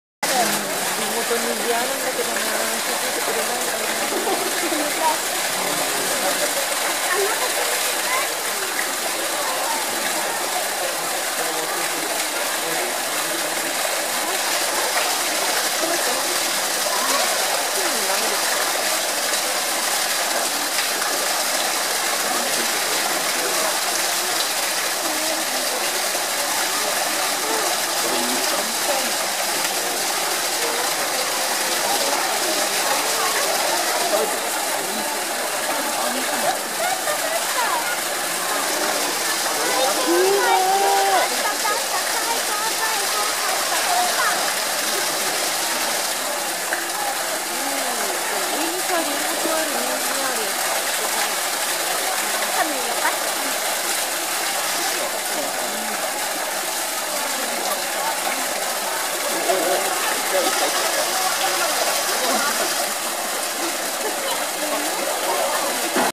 ハチミツペロペロ
おびひろ動物園のホッキョクグマのイコロ。
この日は、あちこちにハチミツが塗られていました。
ペロペロ舐めるイコロの姿が可愛いです。